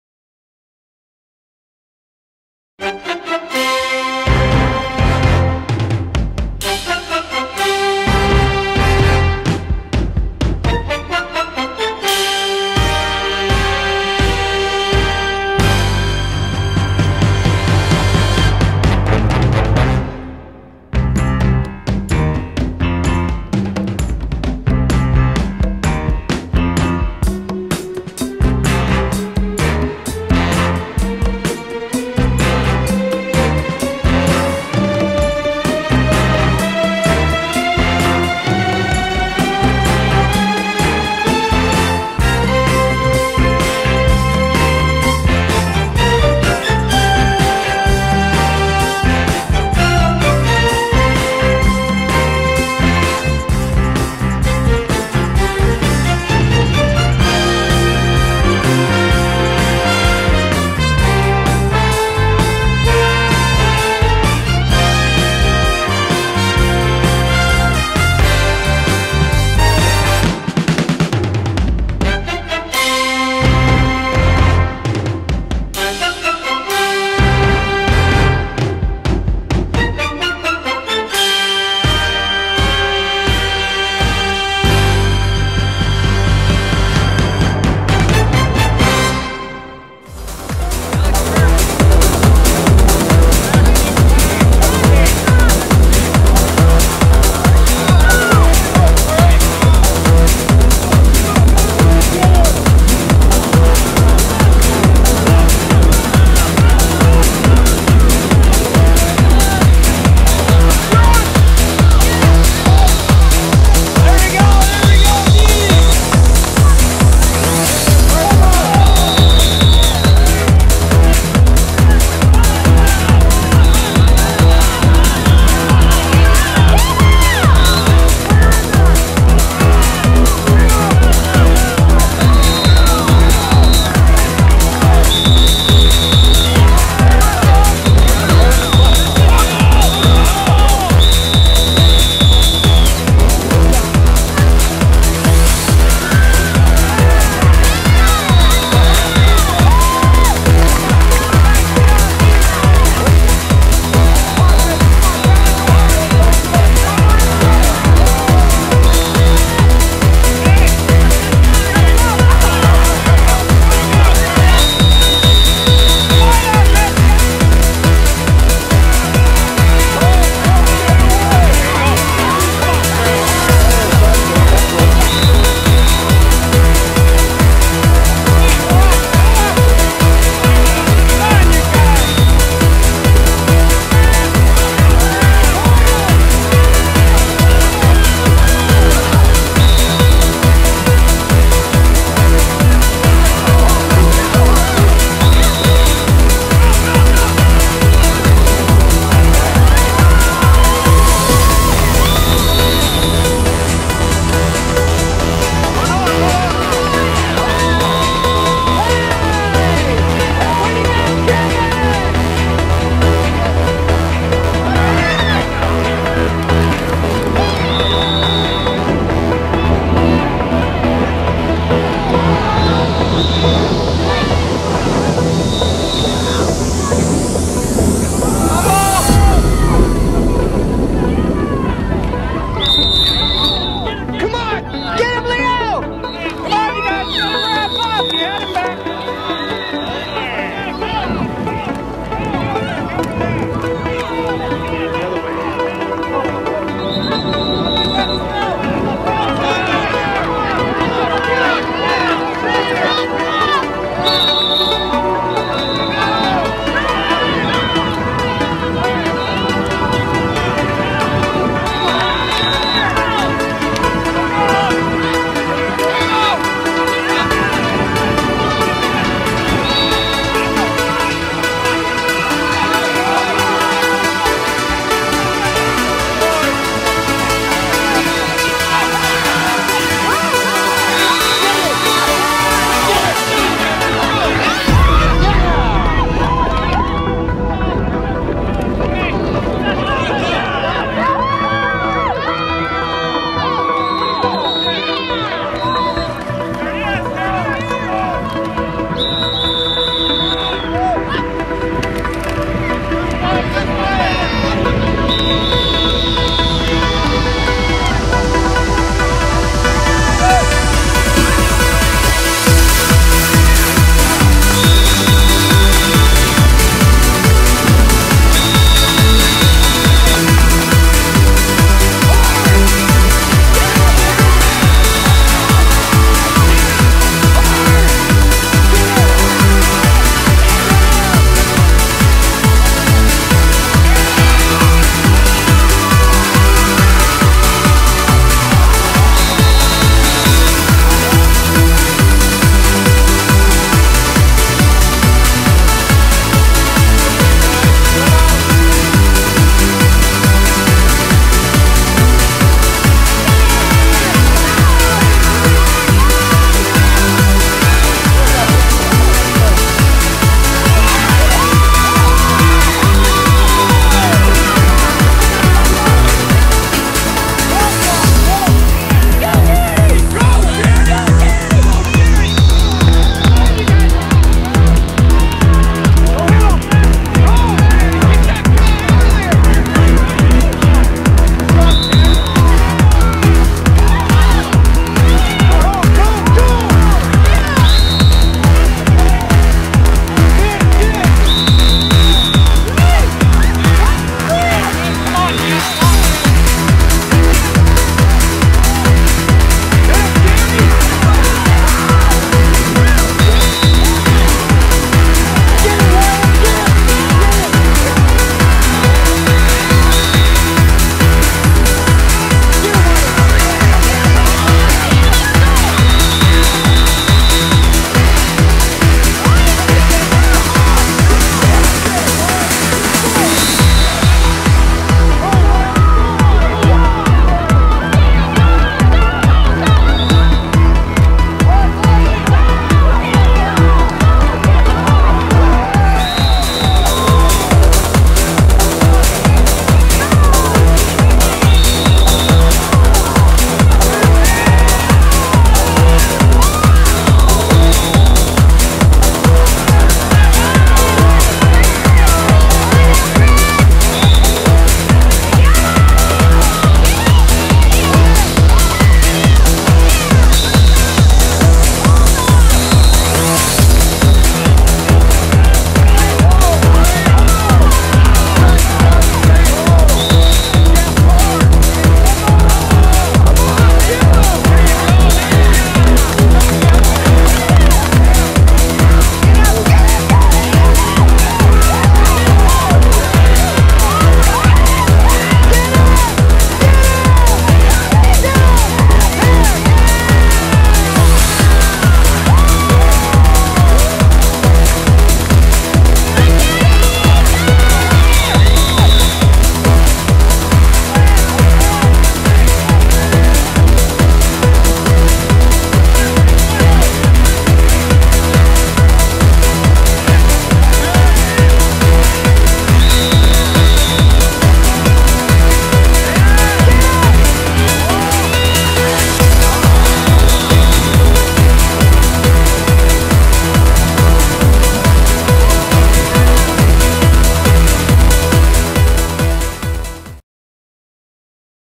100408 Deacons Football
The Deacons are back for their greatest challenge of the 2008 football season: a matchup with the undefeated Browns.  Who will rise to the top after a grueling 60-minute game?  Check out the video to find out...